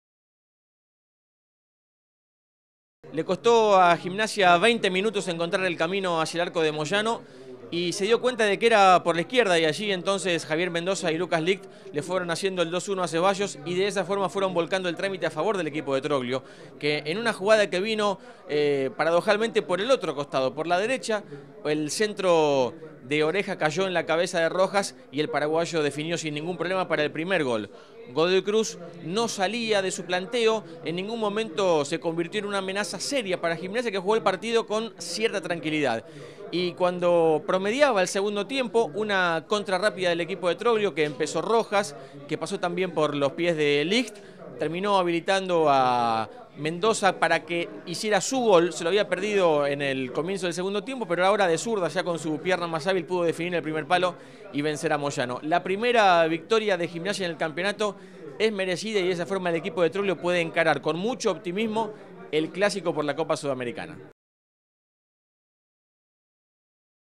Análisis. Gimnasia LP 2 - Godoy Cruz 0. Fecha 5. Primera División. FPT.
El análisis de Fernando Salceda.